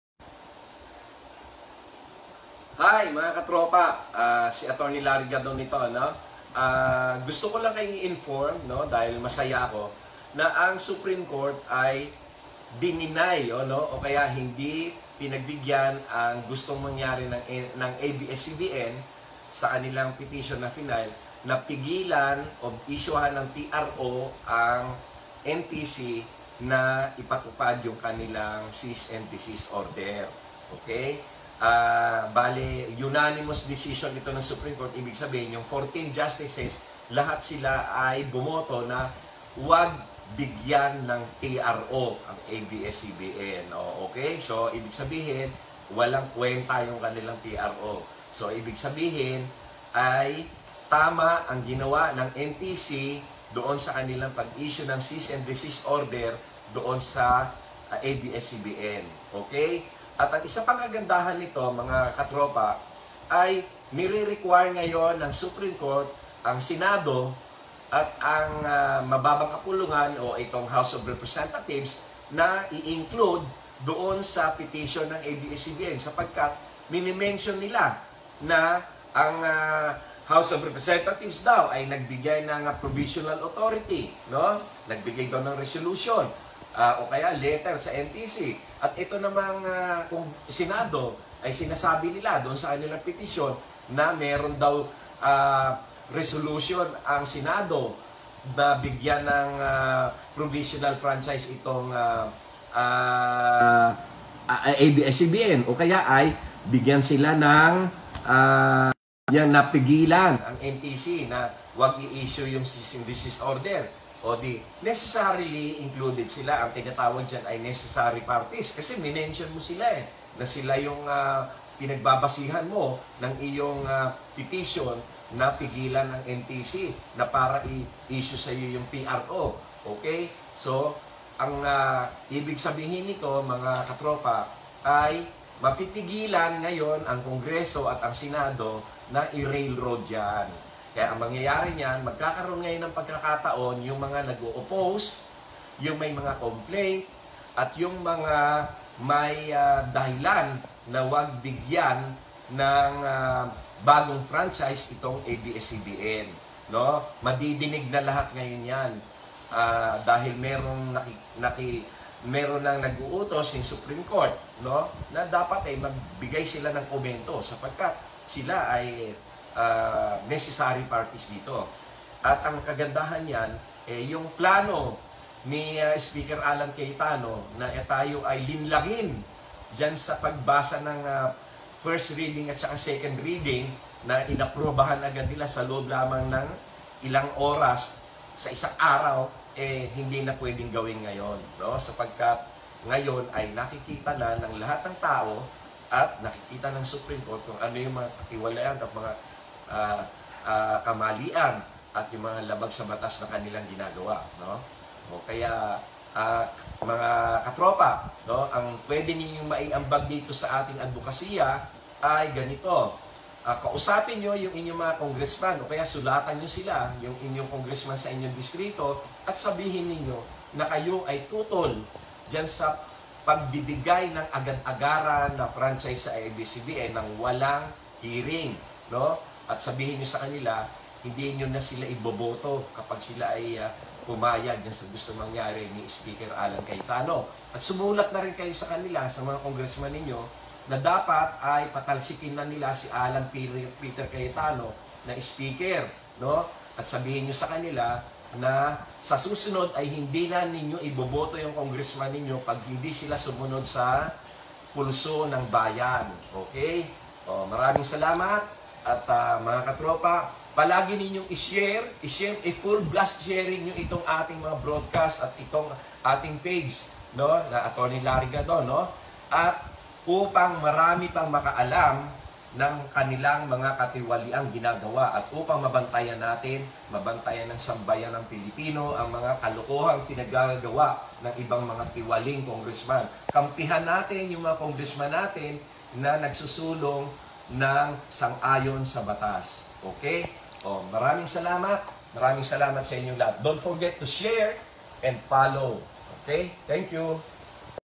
THE SUPREME COURT VOTED UNANIMOUSLY TO BLOCK THE TRO FILED BY ABS CBN( ATTY.LARRY GADON)/MARIDALZ
***VideoCreditTo: Atty.Larry Gadon